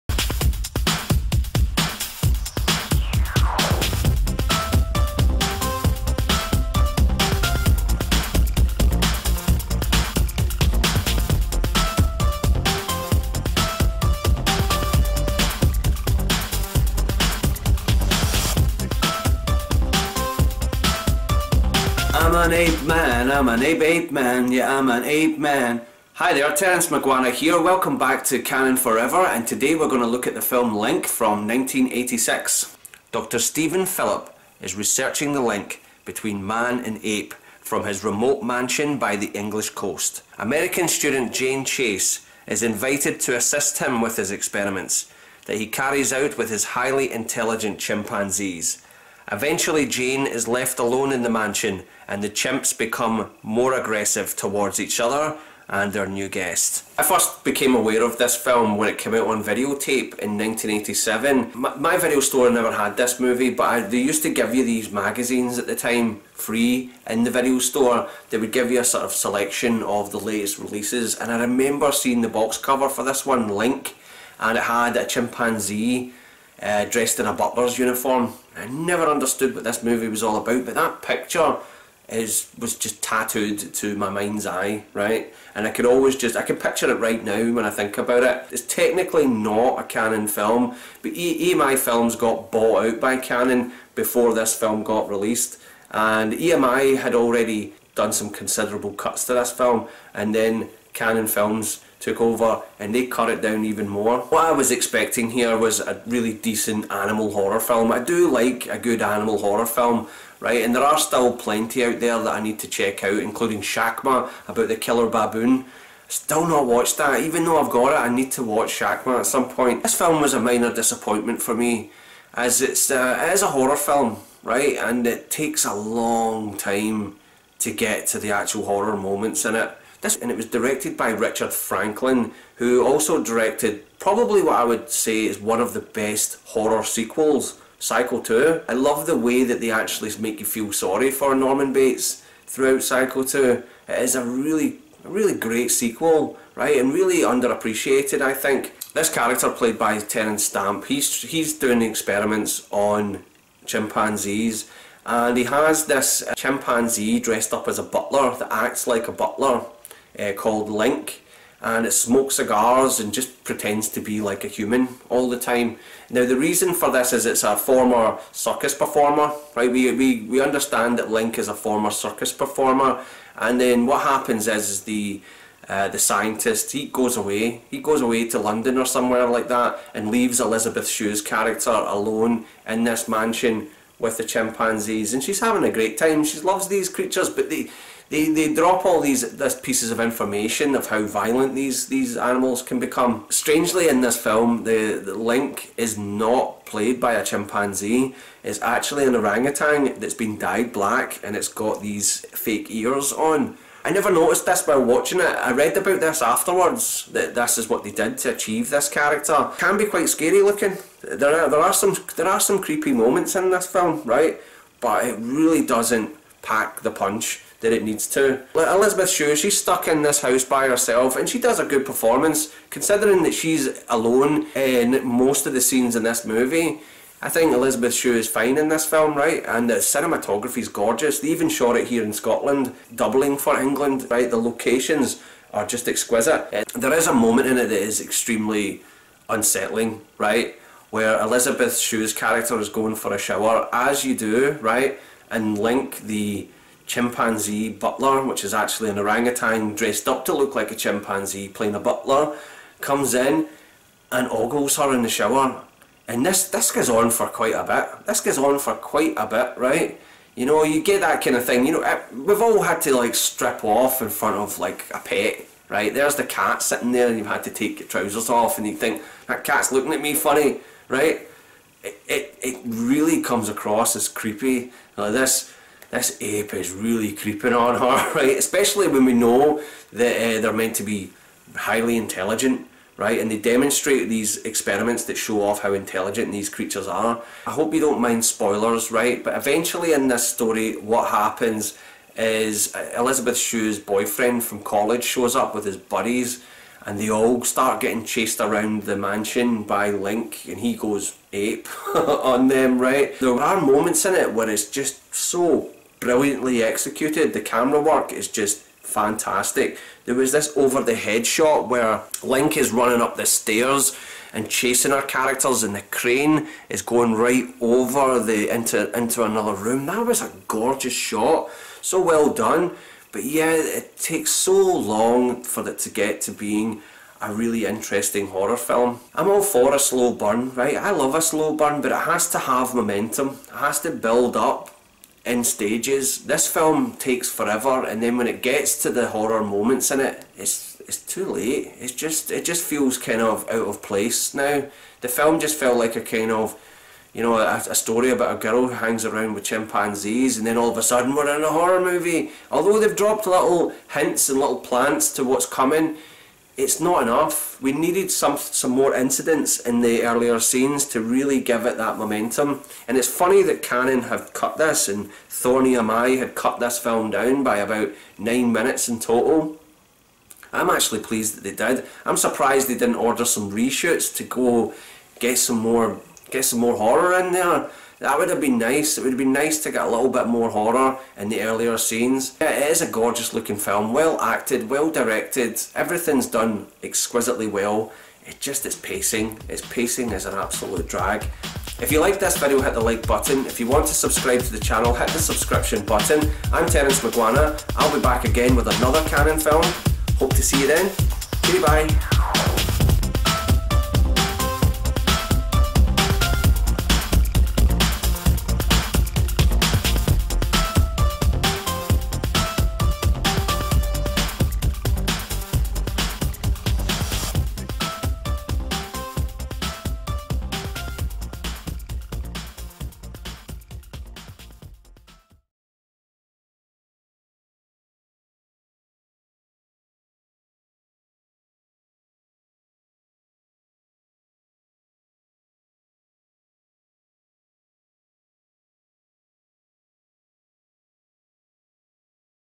Link 1986 CANNON FOREVER EPISODE SIX Monkey Horror Film Review Elisabeth Shue
Here is my review of Link from 1986
part of a nostalgic journey back to the 80's when Cannon Films ruled the cinema screen

Directed by Richard Franklin
Screenplay by Everett De Roche
Story by Lee David Zlotoff and
Tom Ackermann
Produced by Richard Franklin
Starring
Elisabeth Shue
Terence Stamp
Cinematography Mike Molloy
Edited by Andrew London
Derek Trigg
Production
company
Thorn EMI Screen Entertainment
Distributed by The Cannon Group
Release date
31 October 1986 (U.S.)
Language English
Budget Unknown
Box office $1,720,450 (USA)[1]

source wikipedia